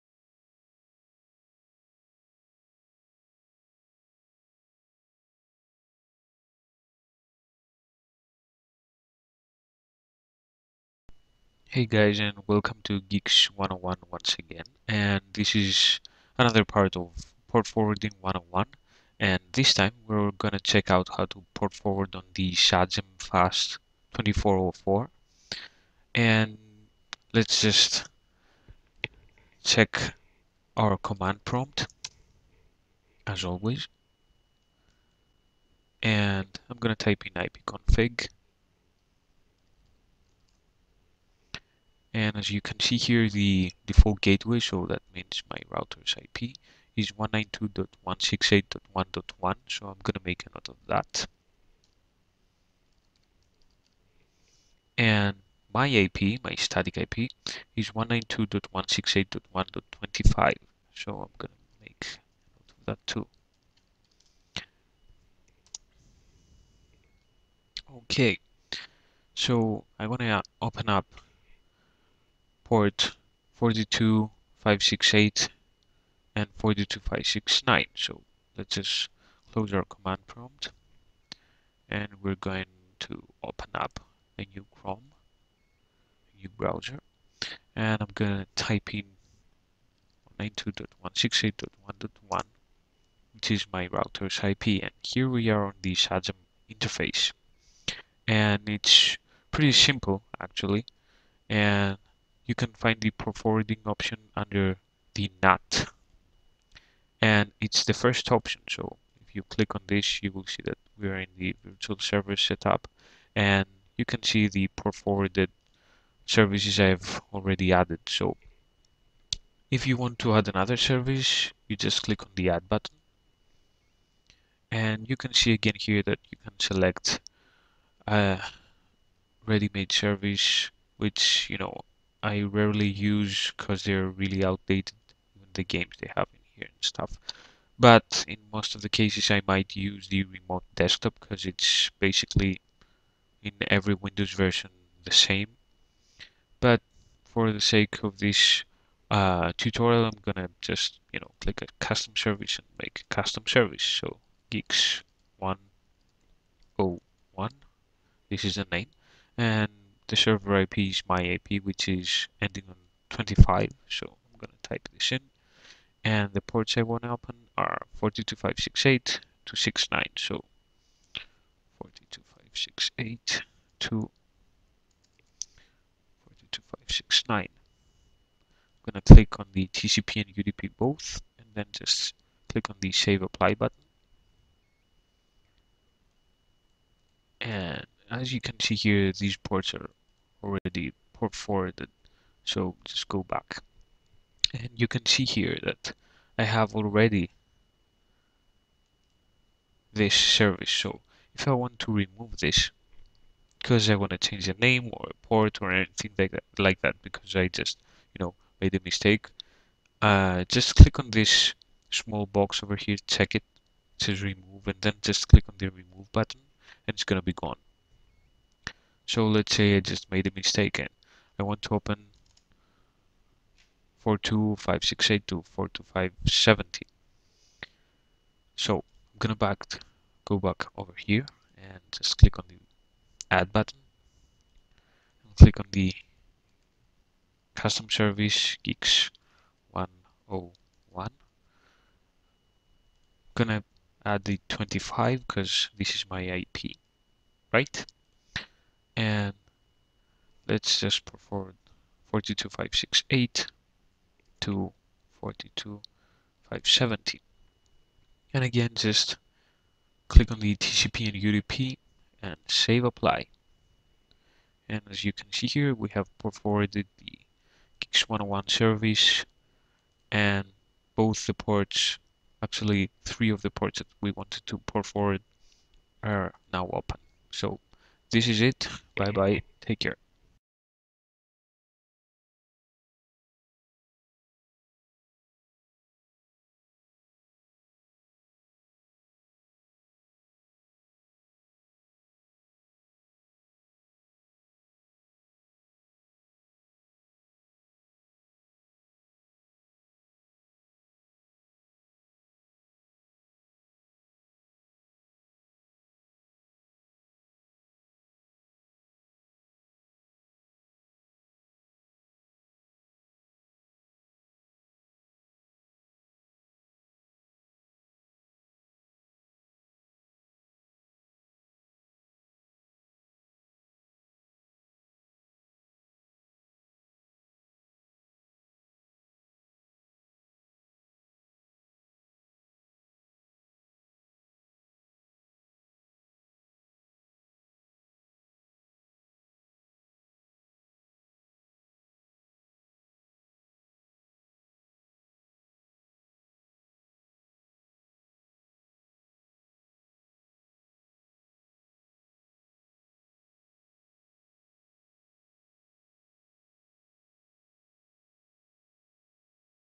Portforwarding 101 - Episode 4: Sagem
Hello everyone! Here we are again with a portforwarding tutorial for the Sagem 2404, we hope you like it and find it helpful. We are looking forward to your questions and we are eager to give any answers and help we can ;) So go ahead and shoot guys.